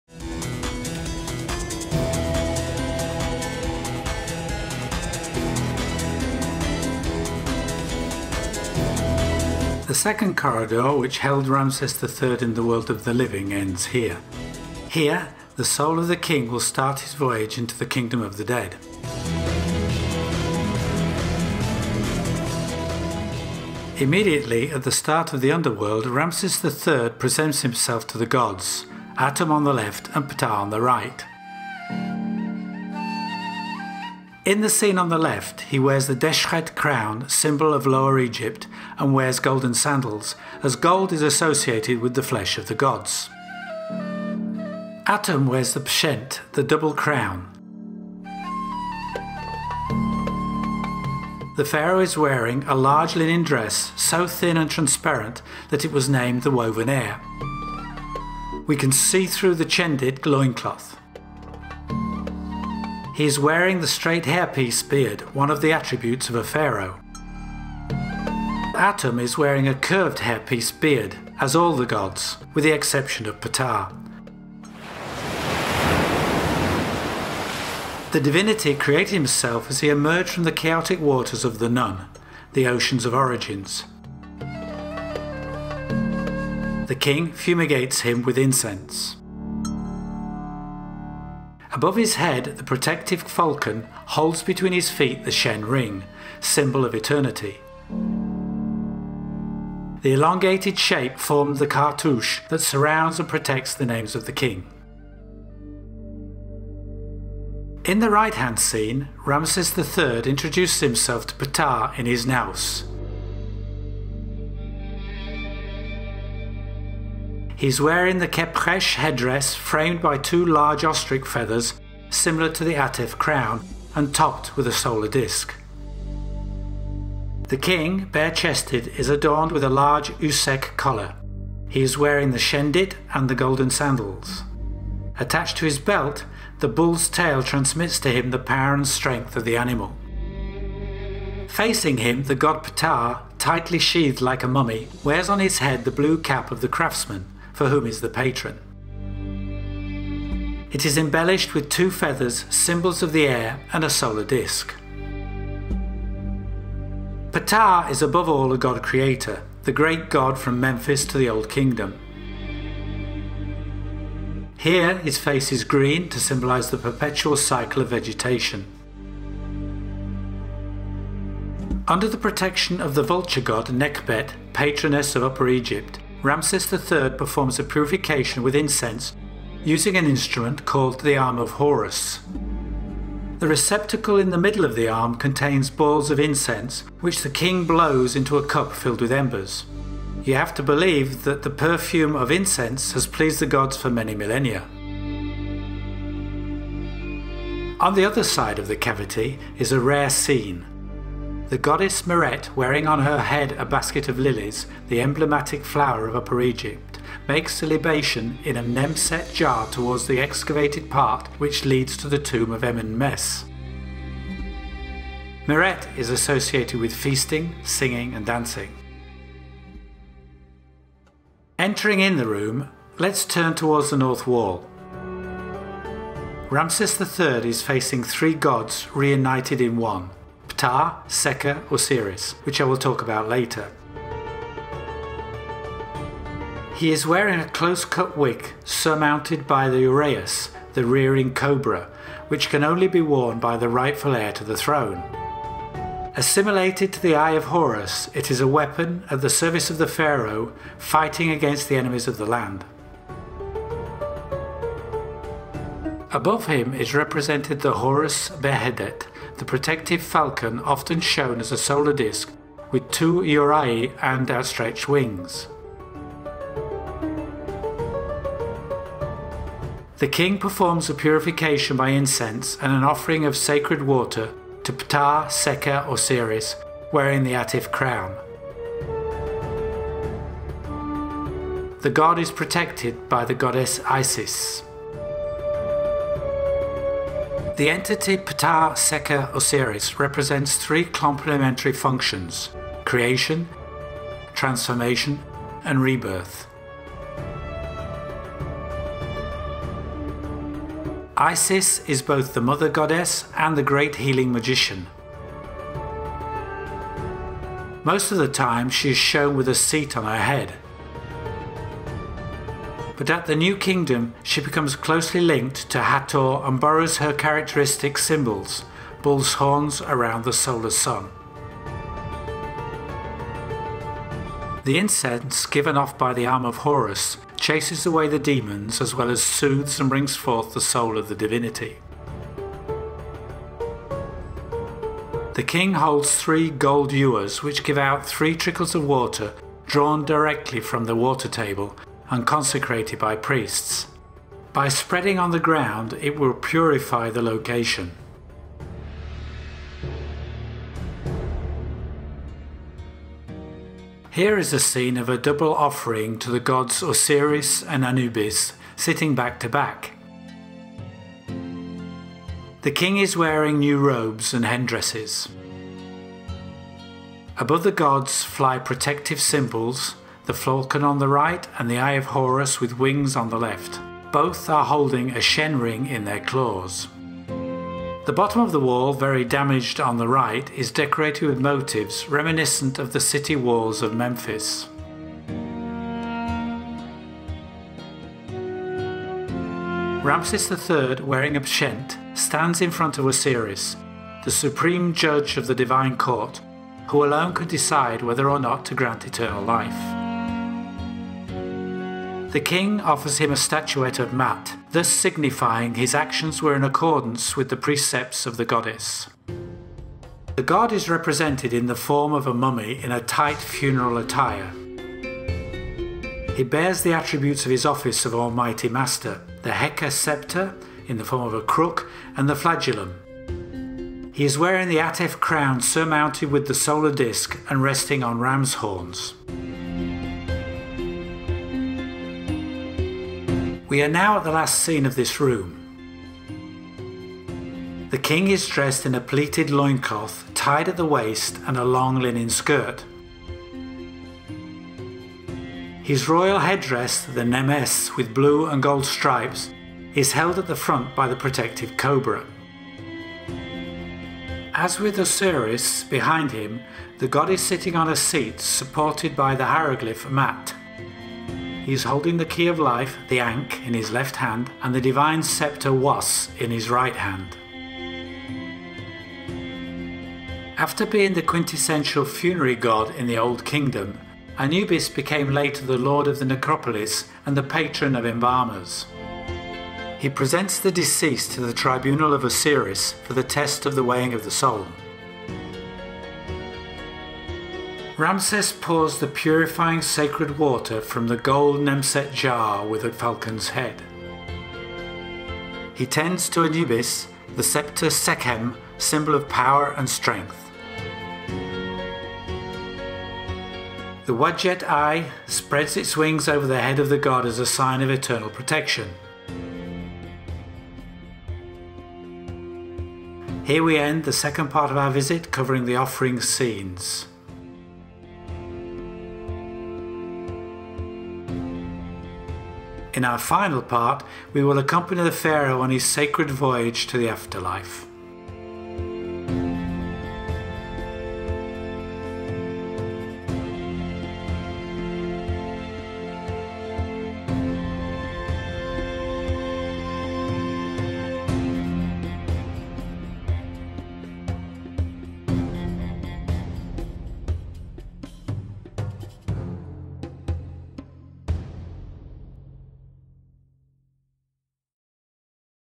EGYPT🔆KV 11🔸Tomb of RAMSES III: Guided visit (Part 2️⃣)
Beautiful intermediate room, between the second and third corridors, where several scenes show the King giving offerings to different gods.